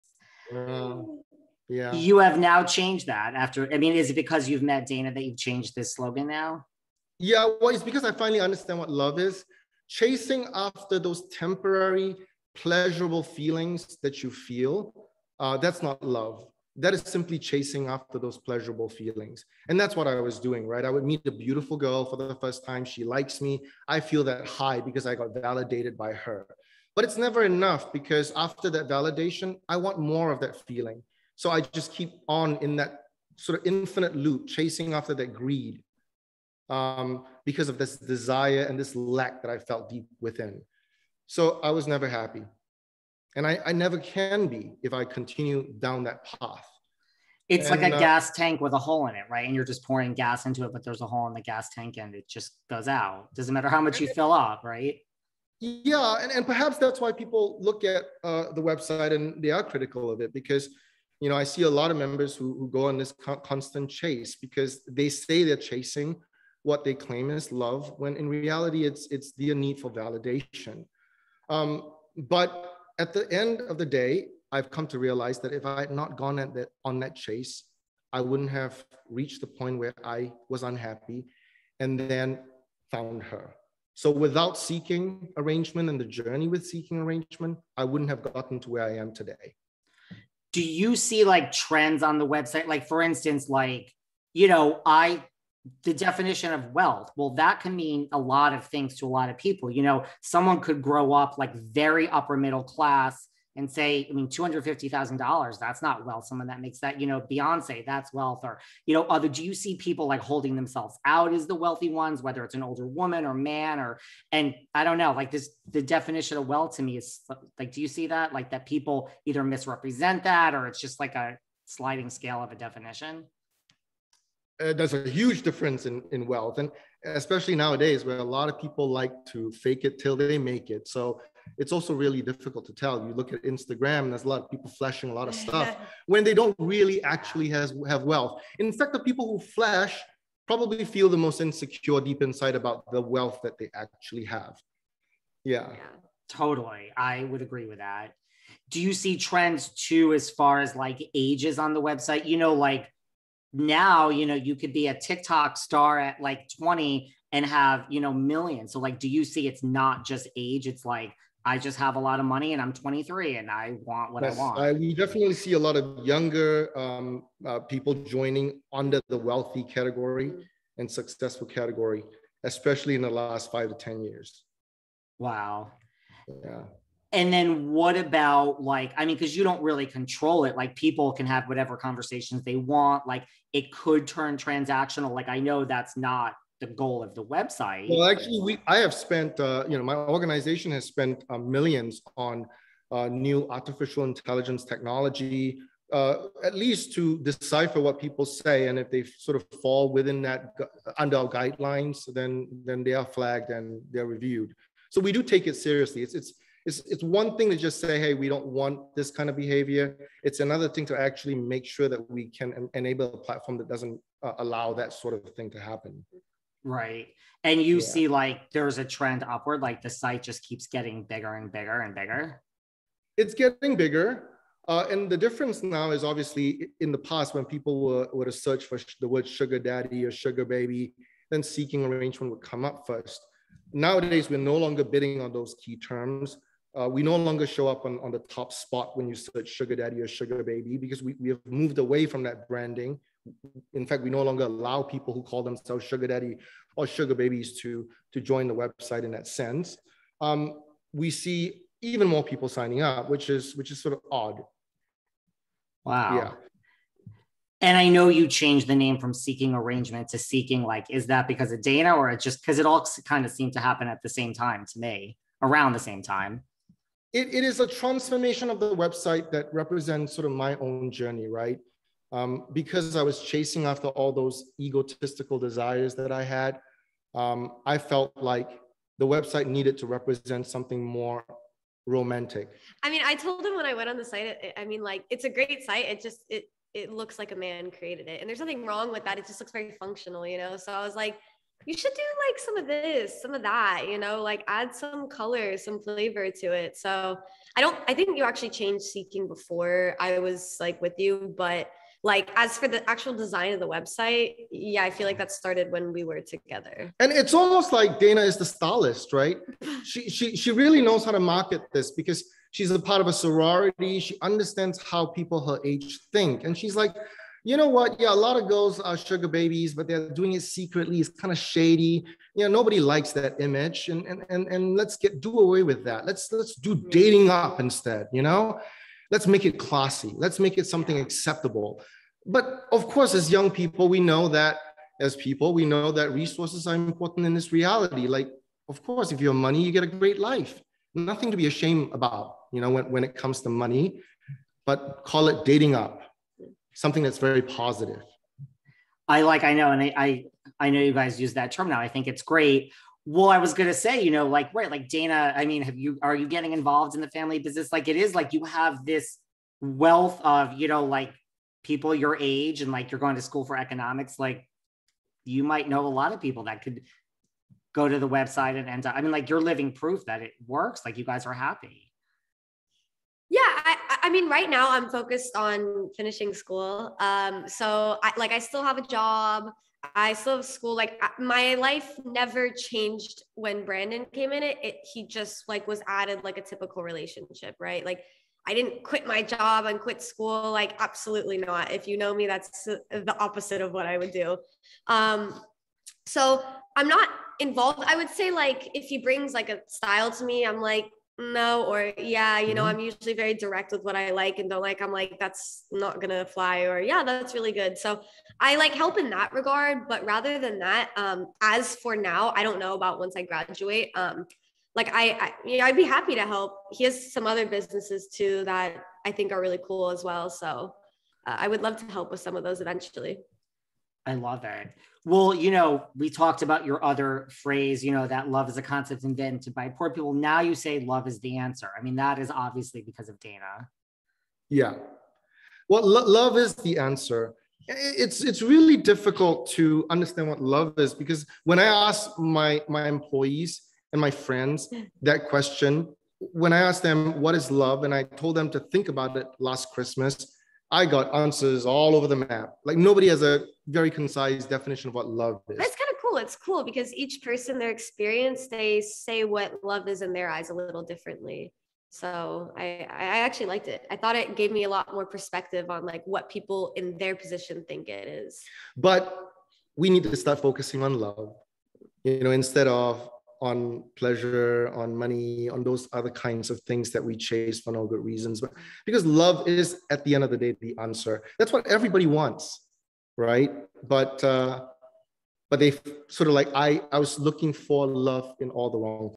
Seeking Arrangement Founder Changes Official Slogan That “Love Is A Concept Invented By Poor People"
Brandon Wade and Dana Rosewall step Behind The Rope.  Brandon, CEO and Founder of famed “sugar dating” website “Seeking”, formerly “Seeking Arrangement”, chats about starting his infamous business, the misconceptions and criticism associated with “Seeking”, and trends he has seen between mature financially successful men and women and younger attractive daters throughout the years. Brandon and Dana chat about their chance meeting on “Seeking” which has not only led Brandon to retract his statement that “love is a concept invented by poor people”, but also drop the “arrangement” from Seeking, rebrand his business - which he hired Rachel Uchitel to assist with, and propose to and now be planning a wedding to Dana. Brandon and Dana talk love - how, where, when they found it and how you can find it too!!  Finally, we analyze some of Hollywood’s most notorious relationships determining just how much “dating up” plays into the mix - Erika Jayne and Tom Girardi, Scott Disick and Amelia Gray / Sophia Richie, Madonna and her Dancer Boyfriend and many more!